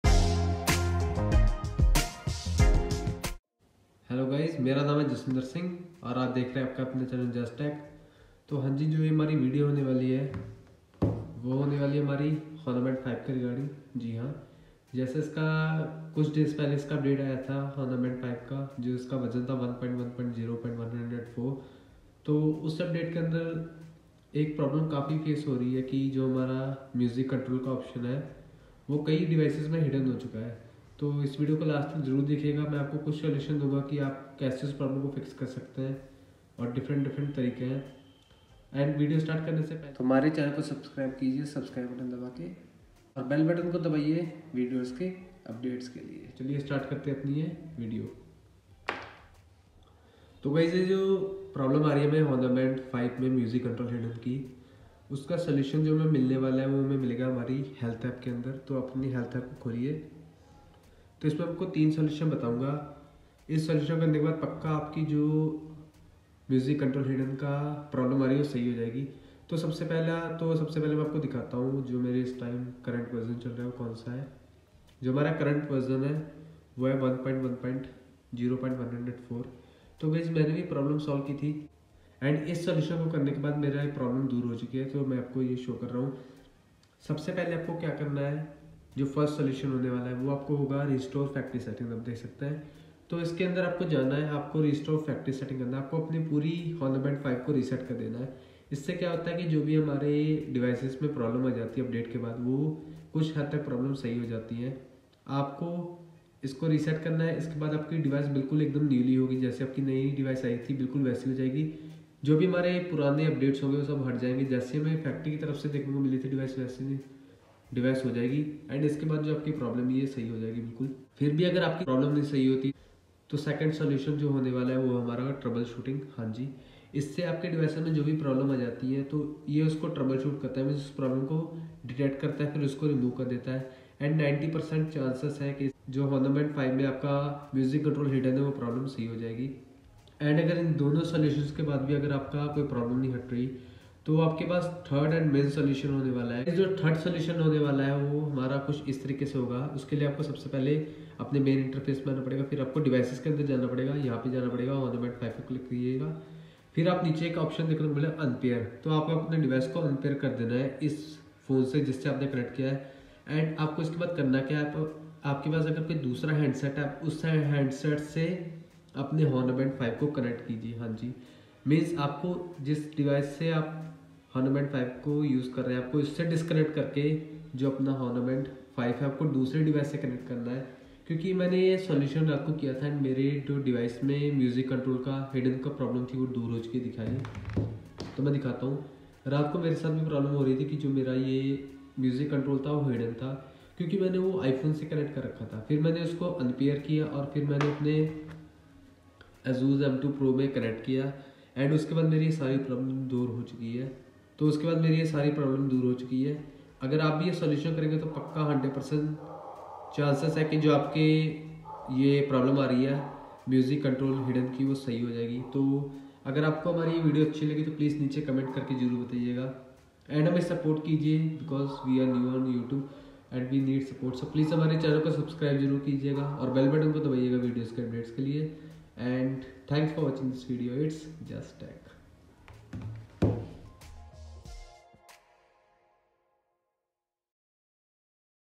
How To Fix Music Playback Control Issue! On Honor Band 5 | Version 1.1.0.104 | Jass Tech
Honor Band 5 got an update last week . It was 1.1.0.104.  In which there was a major problem regarding music control issue . So in this video I am going to tell you 3 solutions to solve this problem. After this The music control via health app will work very smooth and it will not lagg . And now it is working properly.

Category
Science & Technology